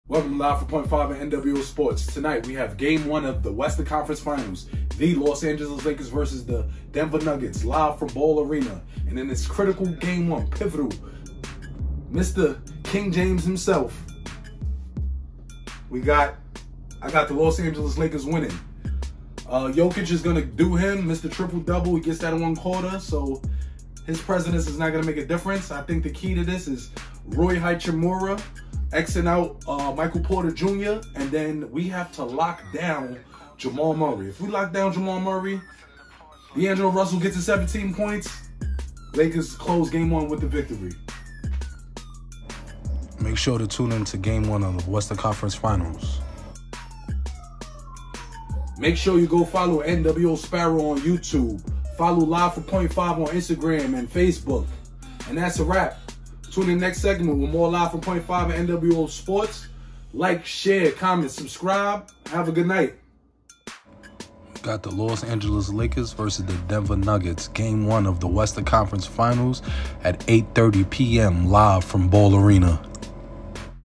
Lakers vs Nuggets game 1 2023
Hey yall whats up im back with another prediction. Lakers vs. Nuggets prediction, odds: 2023 NBA Western Conference finals picks, Game 1. Ball Arena is the center of the basketball world on Tuesday evening. The Denver Nuggets welcome the Los Angeles Lakers to Colorado to begin the 2023 Western Conference finals with Game 1. The Nuggets have home-court advantage in this 2023 NBA playoff series after earning the No. 1 seed, and Denver defeated the Minnesota Timberwolves and Phoenix Suns to reach this point. The Lakers knocked off the No. 2 seed Memphis Grizzlies before defeating the Golden State Warriors. Jamal Murray (illness) is listed as questionable for the Nuggets. Tip-off is scheduled for 8:30 p.m. ET.